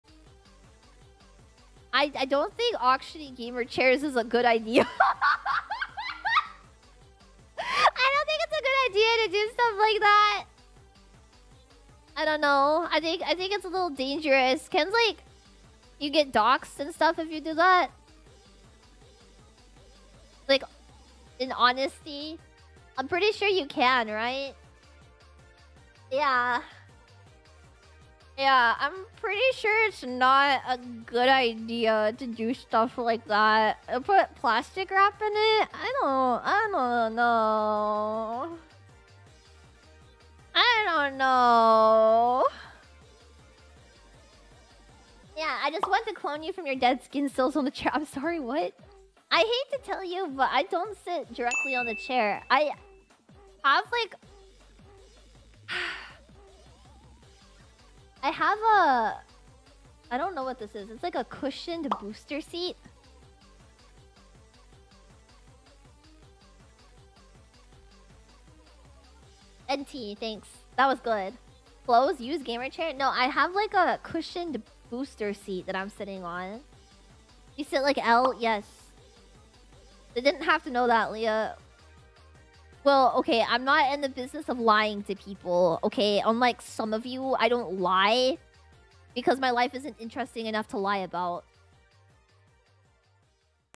Lia Thinks About Selling Her Gamer Chair
Since its popular, the topic of Vtubers selling their gaming chairs came up.  It takes a full 0.5 seconds for Lia to disregard the idea especially knowing how her chat behaves.  Chat reflects the streamer I guess.

Imagine the smell.

Source Video
NUKED

I will eventually start creating content of my own to post.